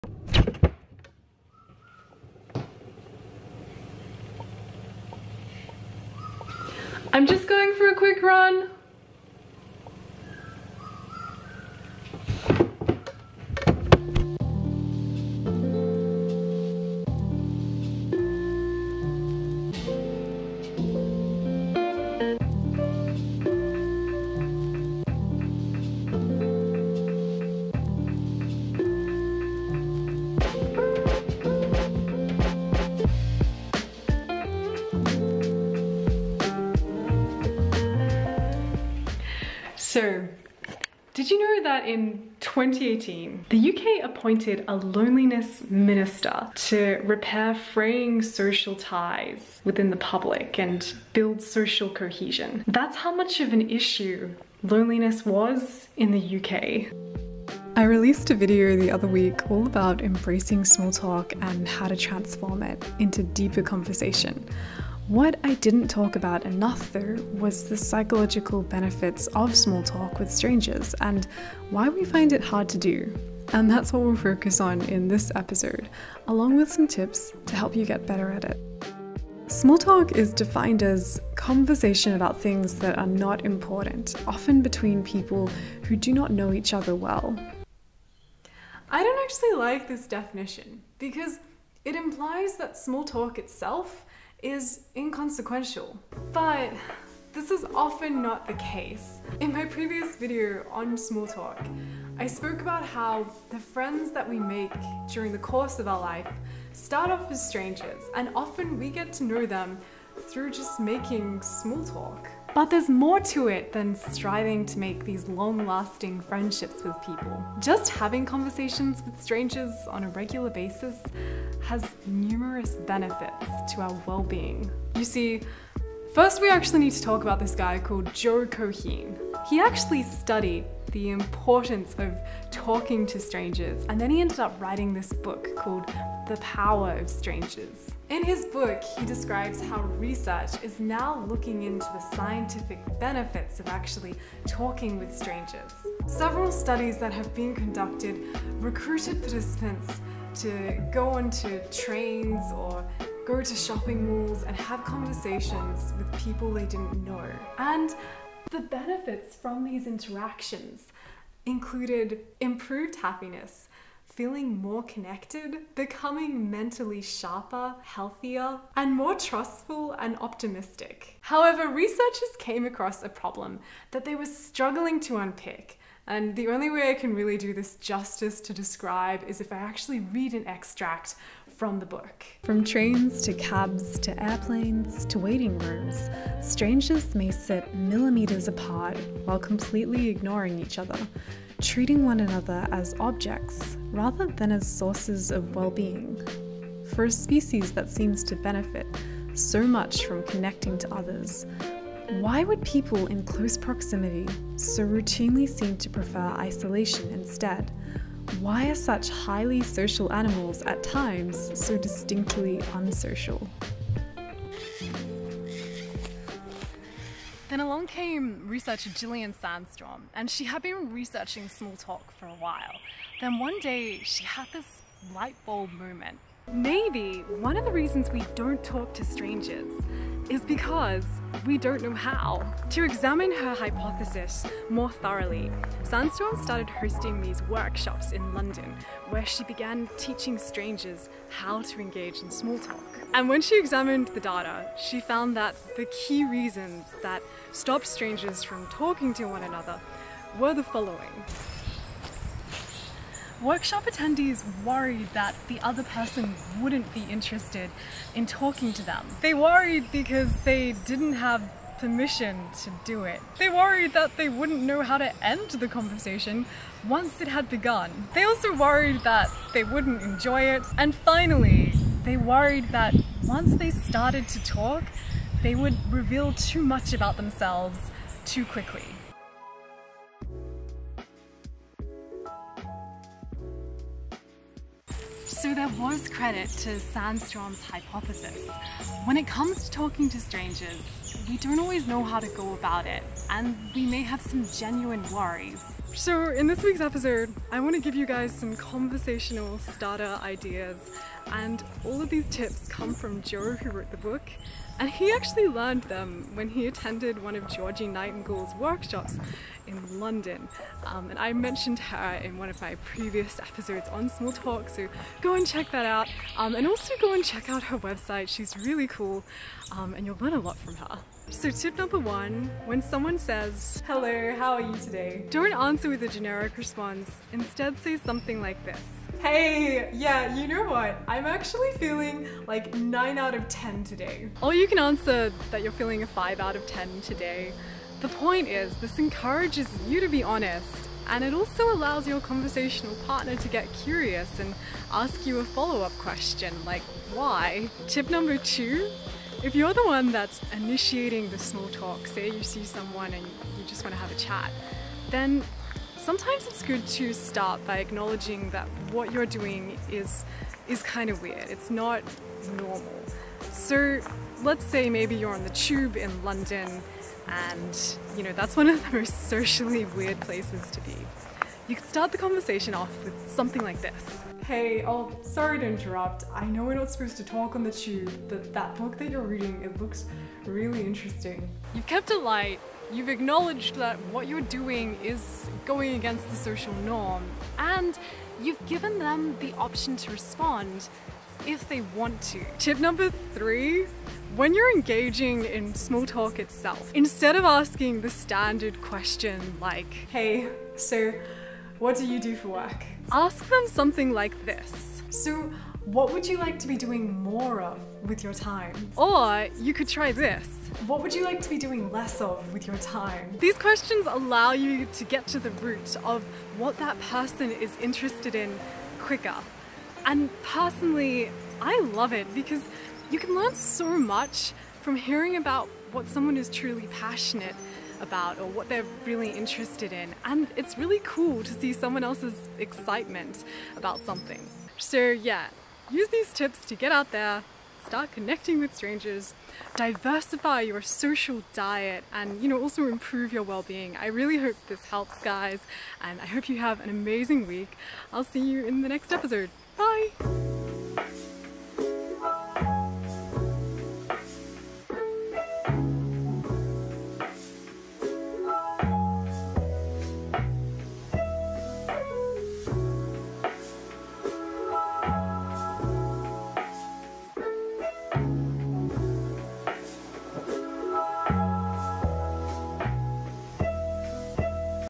Why we avoid small talk and tips to become a better conversationalist
In this week's episode, I break down the benefits of small talk, and research that helps us understand why we find small talk difficult. I also provide some tips for improving your conversation with strangers.

✍️ Kathleen x